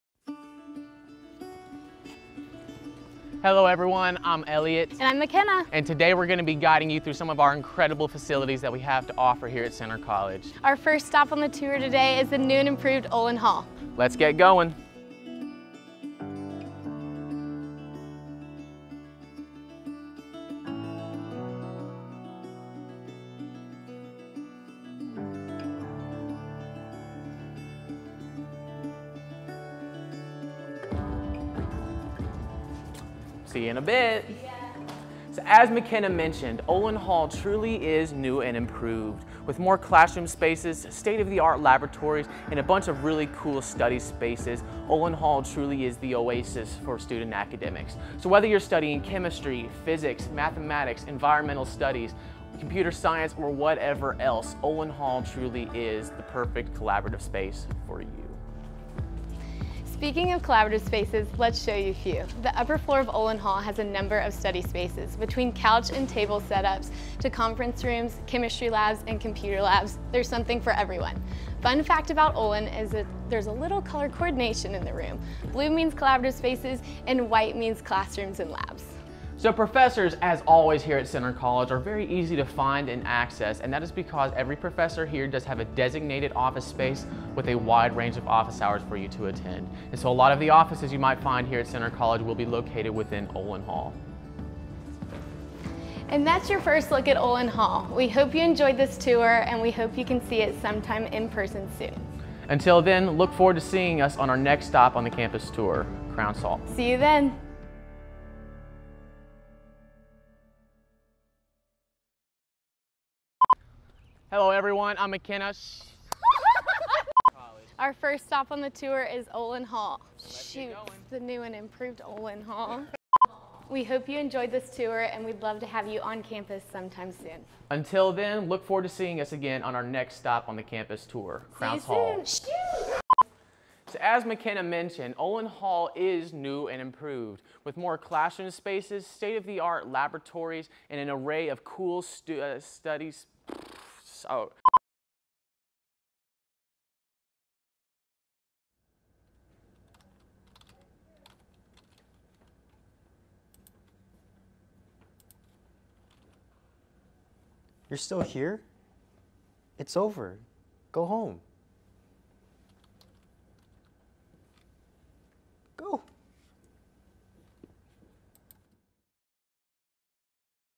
Centre Snapshots: Olin Hall
Meet your hosts Elliott and McKenna as they take you around our new an improved science and mathematics center; Olin Hall. This is a great space on campus for fostering student collaboration and promoting student lead research. We hope you enjoy this quick look, and keep an eye out for more tours that are soon to come!